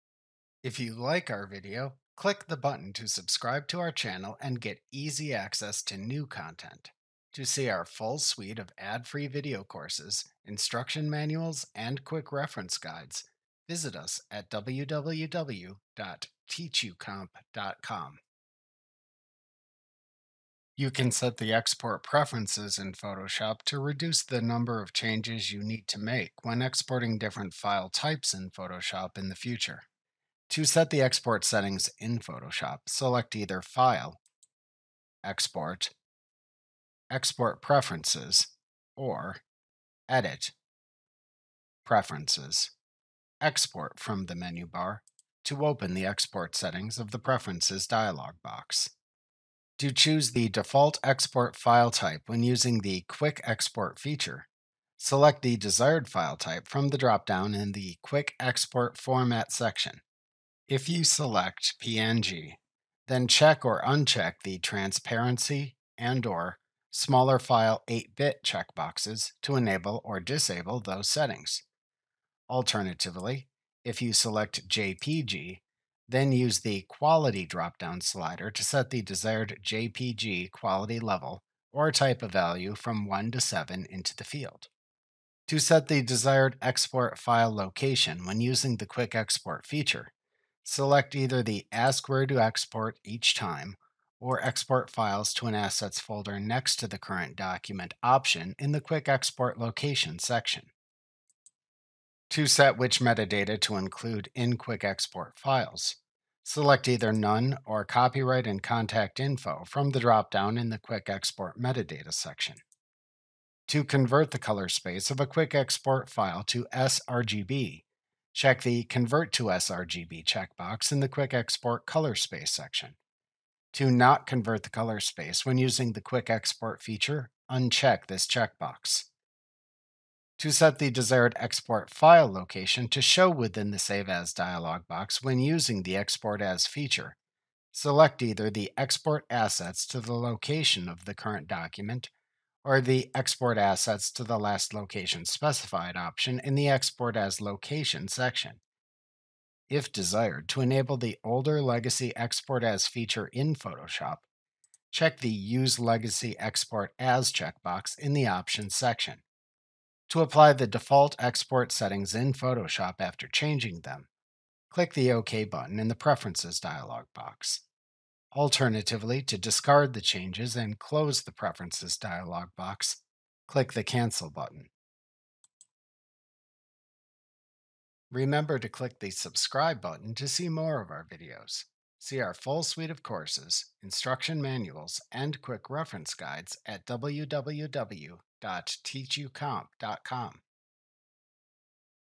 Learn How to Set Export Preferences in Adobe Photoshop: A Training Tutorial
This Photoshop tutorial video shows you how to set export preferences in Photoshop. This video is from our complete Adobe Photoshop training, _Mastering Photoshop Made Easy_ ™.